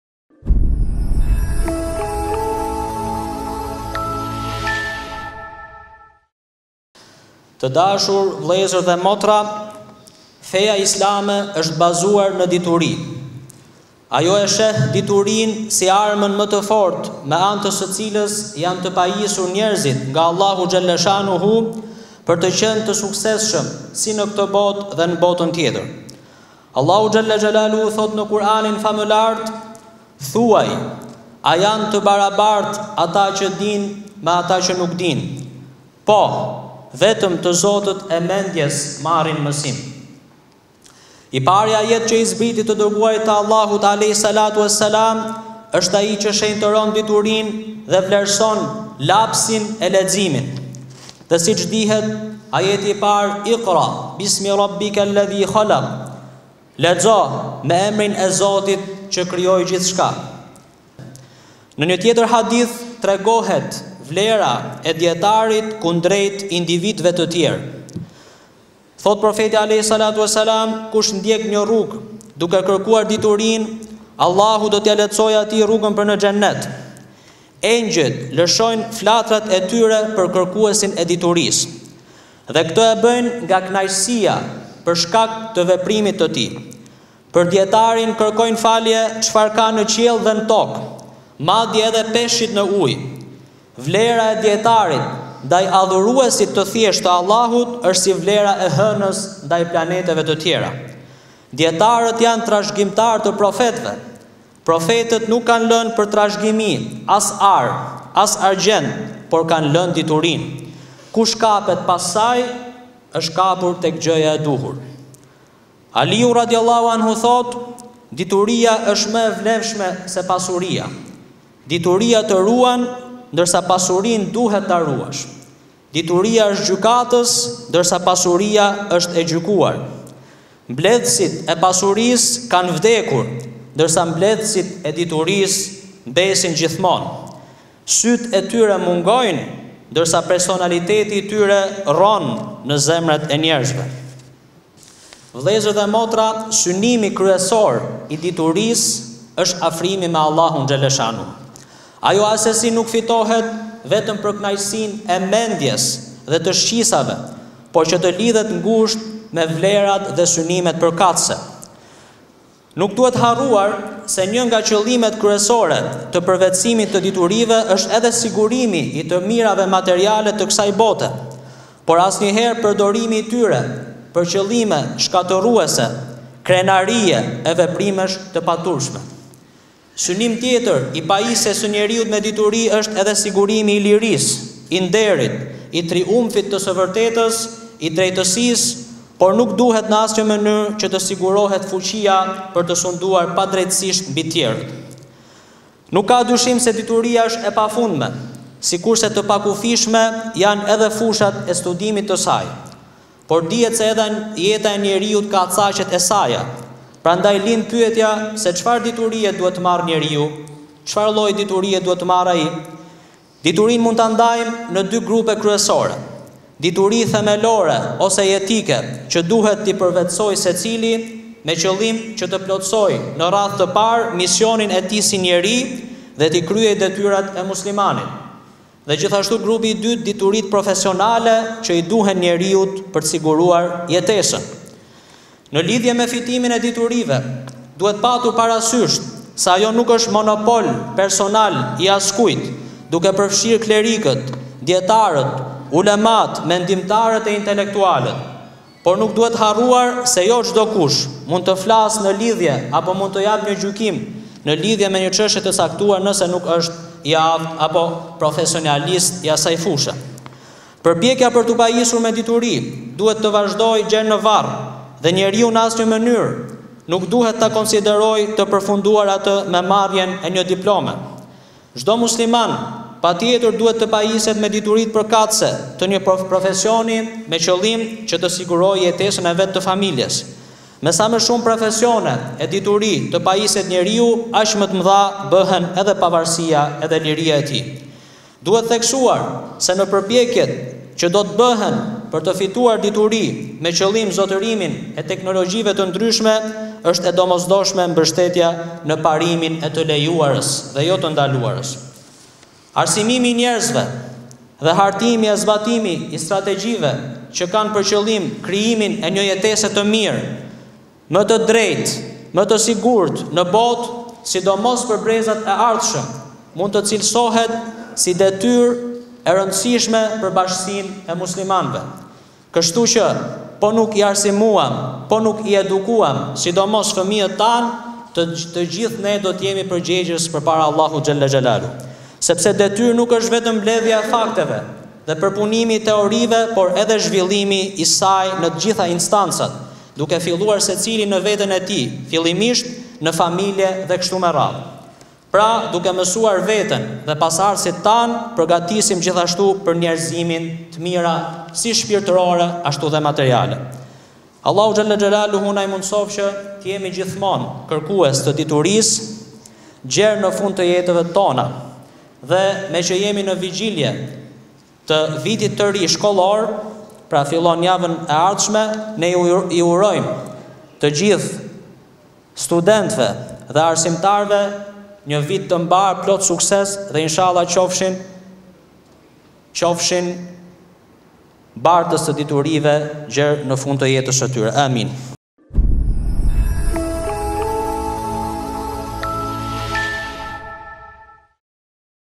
Hutbeja e javës - Vlera e diturisë
“Vlera e diturisë”

Hutbeja e javës nga Hafizi, Enes Hyka.

Adresa: Rr. George W. Bush Nr. 50

Tirana, Albania